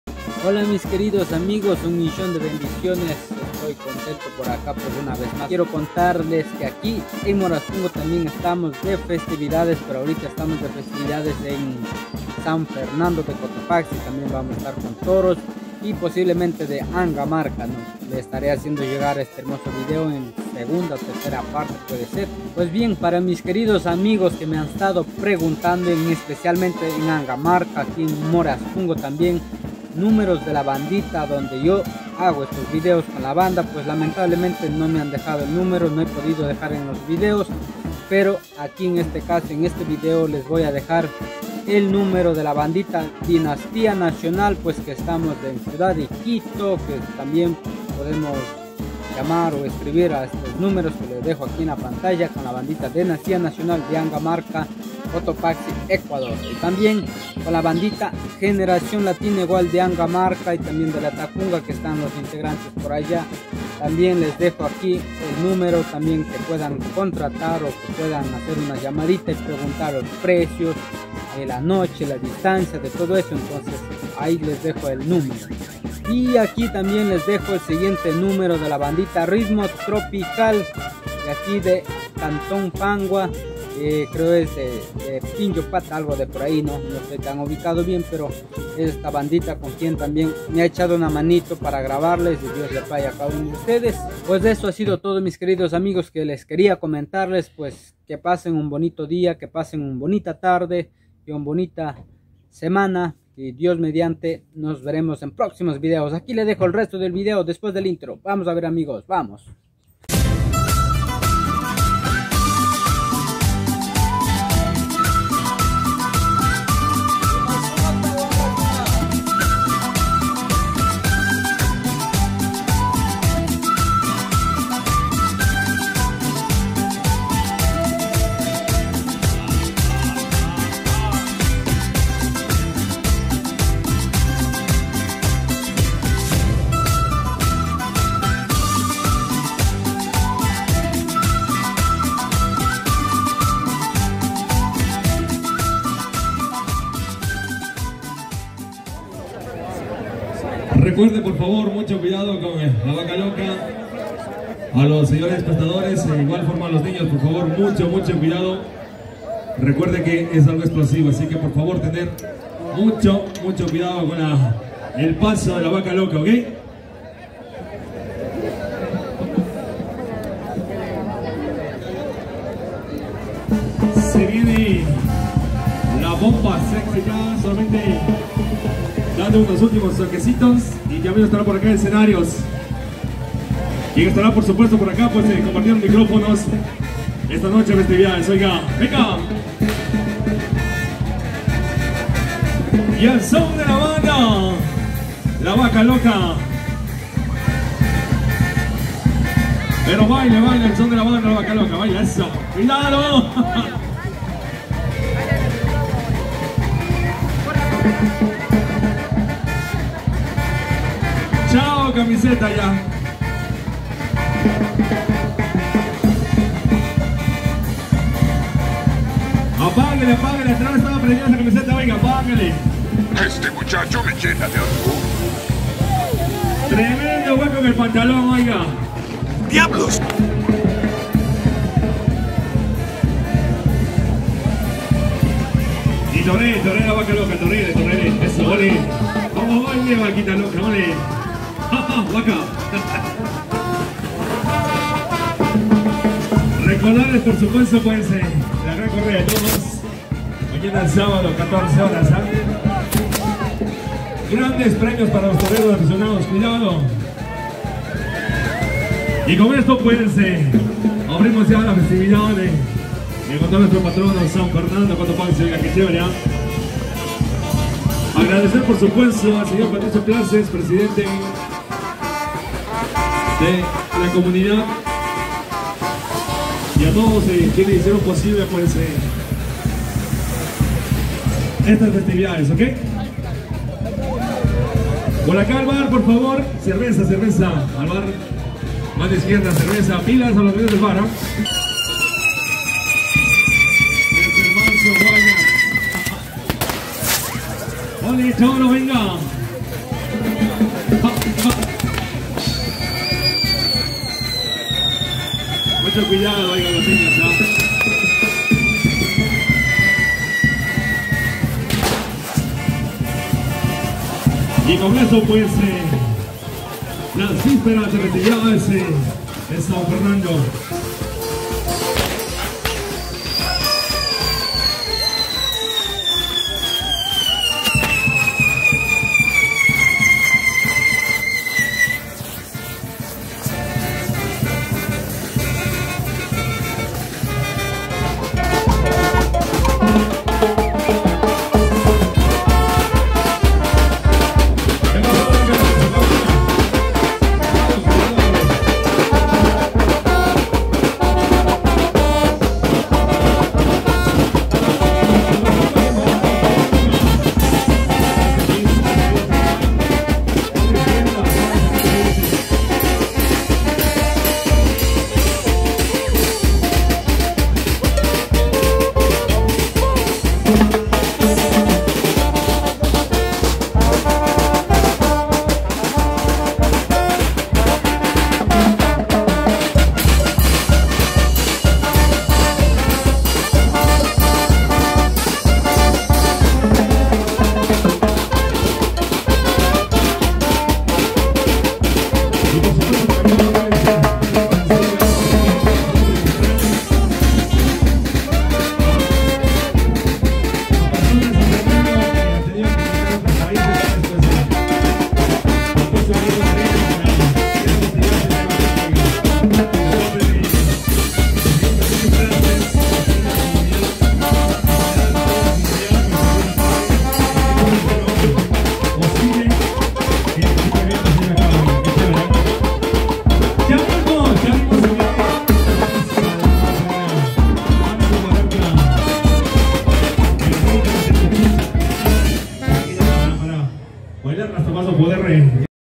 La quema de la vaca loca San Fernando de Moraspungo
Gracias a todos X su Apoyó a Qui les dejo los números de  teléfono  de las Bandas musicales y un poco de las festividades de  San Fernando De Moraspungo cotopaxi Ecuador 🇪🇨